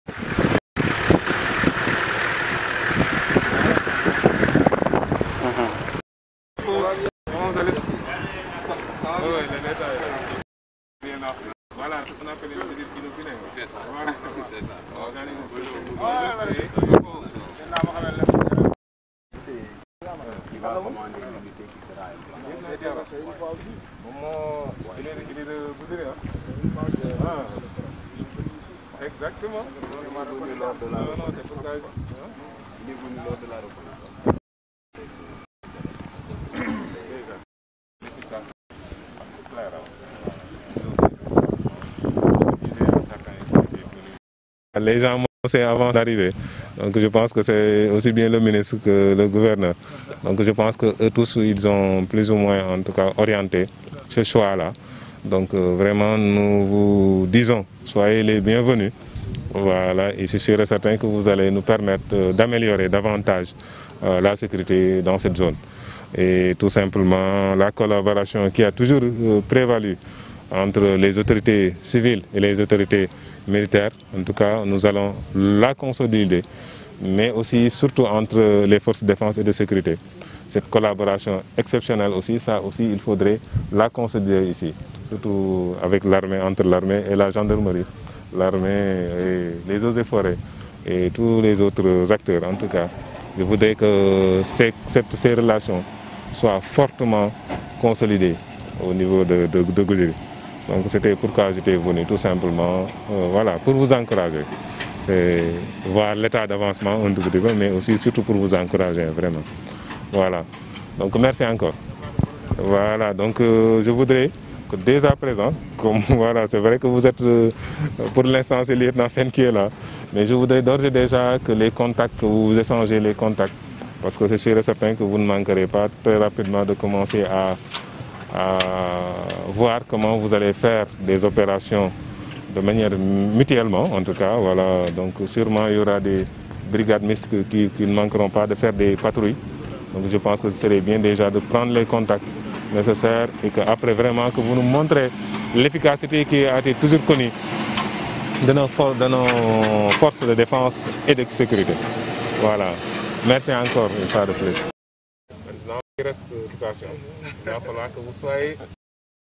Goudiry/Sécurité : La visite du Préfet Ahmadou Coumba Ndiaye au Camp militaire de Goudiry
La visite du Préfet Ahmadou Coumba Ndiaye au Camp militaire de Goudiry